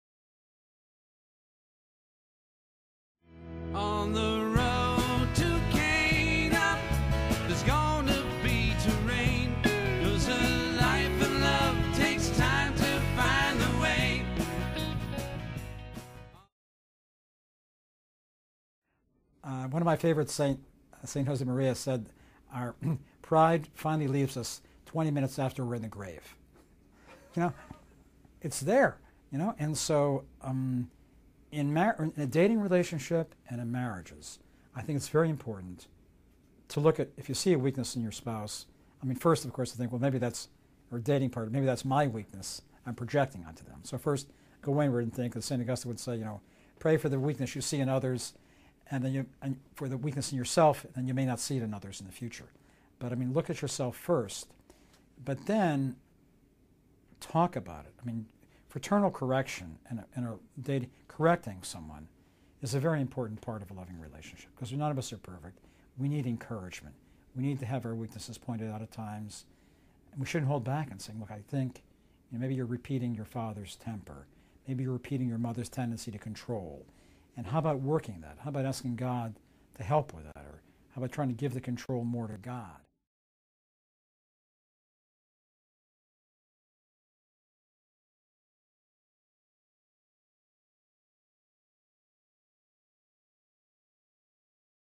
Dr. Richard Fitzgibbons - Discovering Our Weaknesses*
Dr. Richard Fitzgibbons talks about ways to discover our weaknesses on the Road to Cana DVD series.